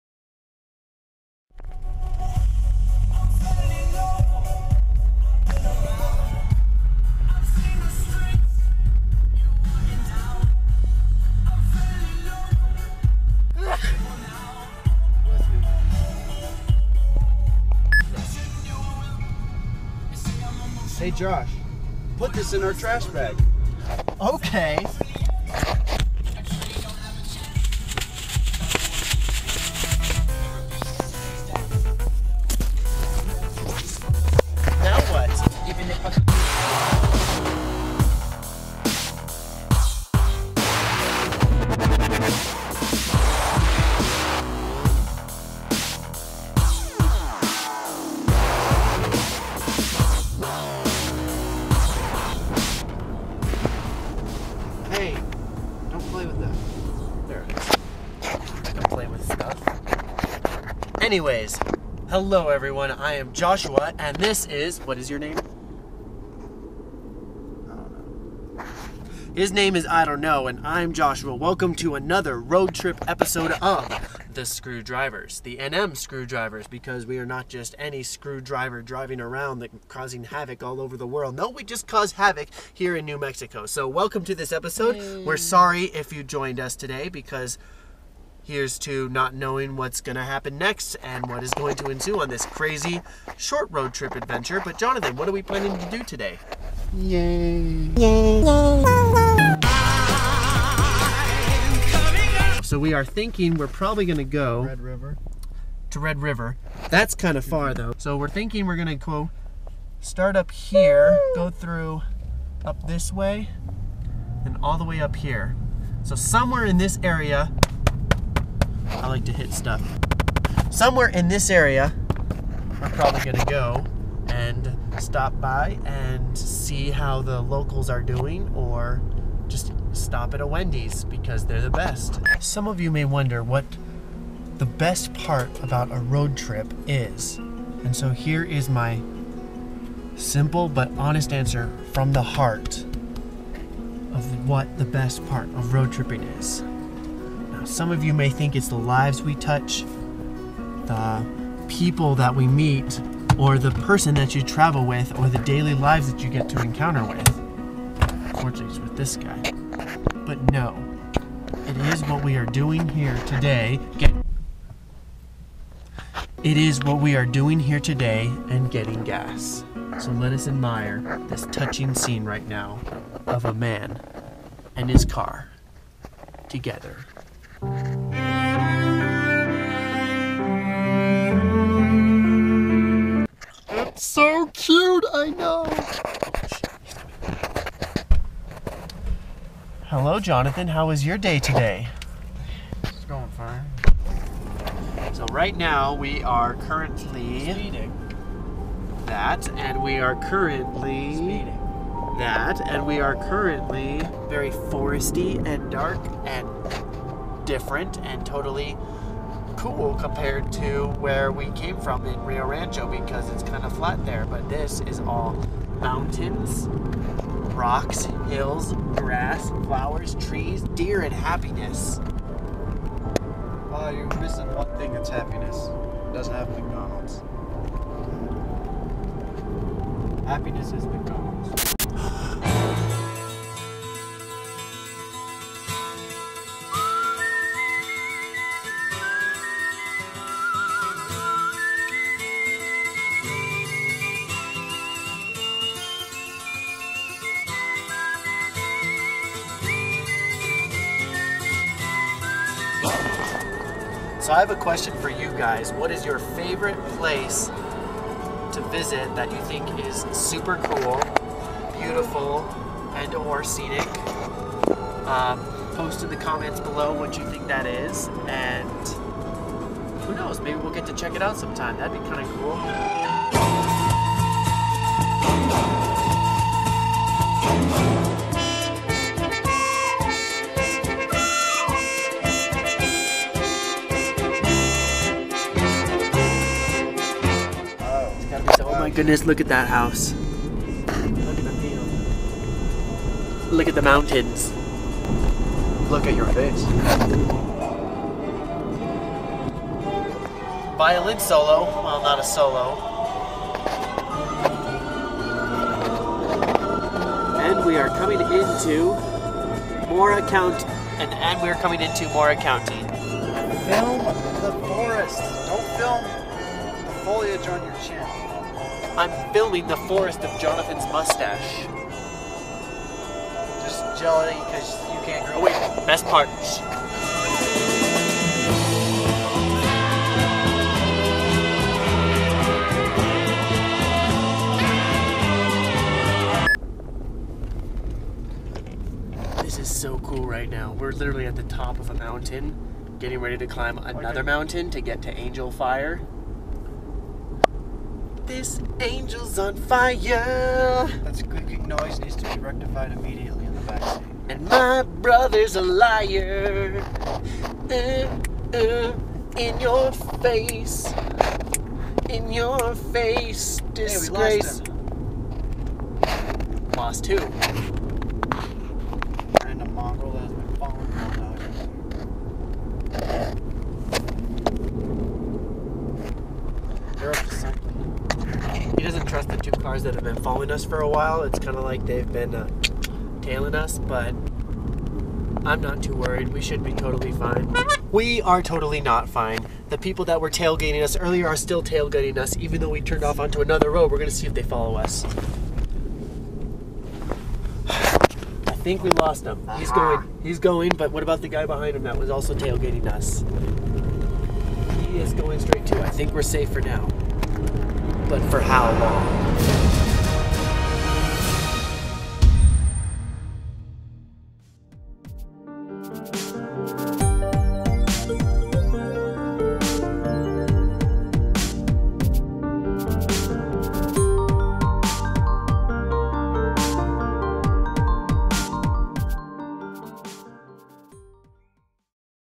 Long Way for a Picnic! - Episode 1 - Flathead Friday
In which two of the NM Screwdrivers, Johnny and Josh, take a 200 mile, four hour, roadtrip to the mountains in order to have a picnic in the woods.

Let us know what you guys want to see, and we may use it!
And just like every other time,
We'll see YOU guys, on the other side!